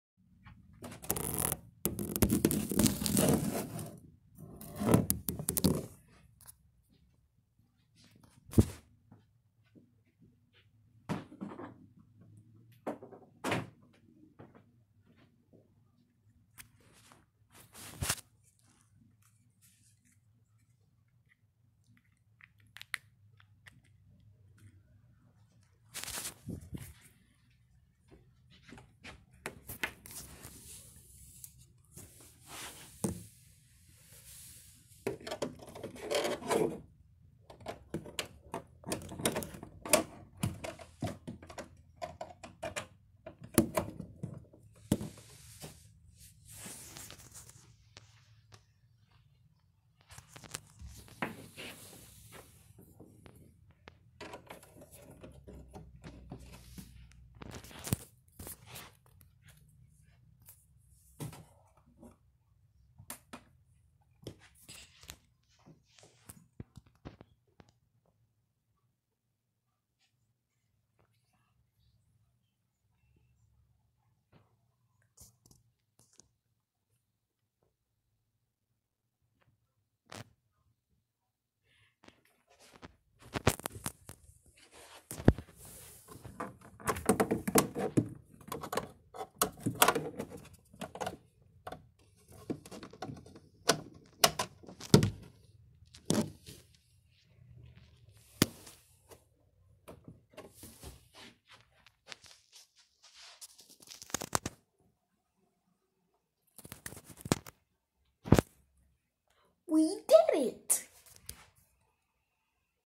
Rare Villain Defeats: The Non Working Signal
when I pluged the back in the cord to the TV the signal that wasn't working washed away from a existence from my TV WDI. Quotes: .